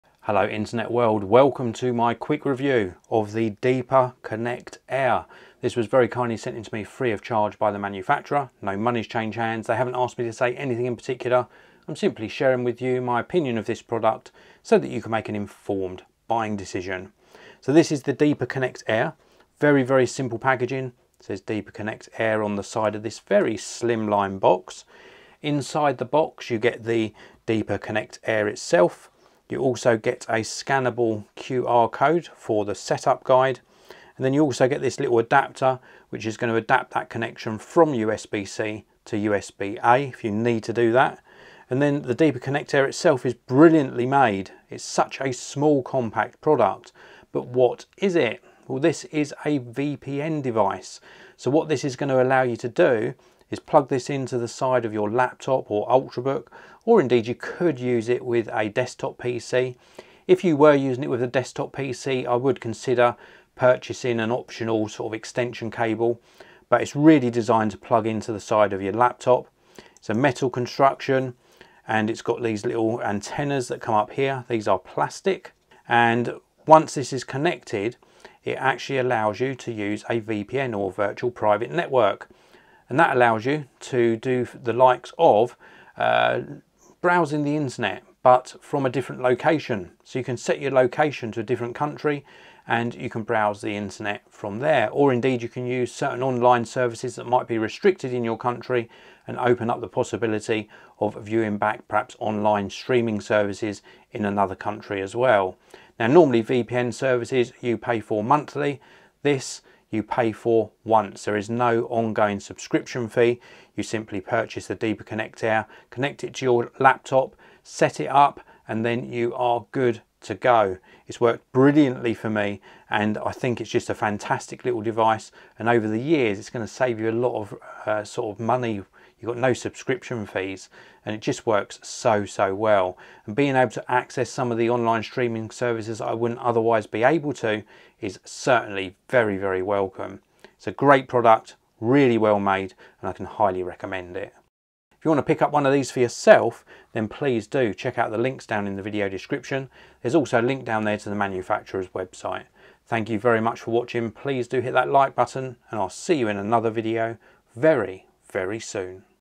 Deeper Connect Air Next Gen Decentralised VPN Router Review
Deeper Connect Air is the world's best VPN router empowered by both decentralized technology and cybersecurity features. It is highly adaptable to mobile scenarios, providing users with a more secure, more private, and fairer internet.
It is highly adaptable to mobile scenarios, providing users with a more secure, more private, and fairer internet.

My Editing Monitor is now out of stock - alternatives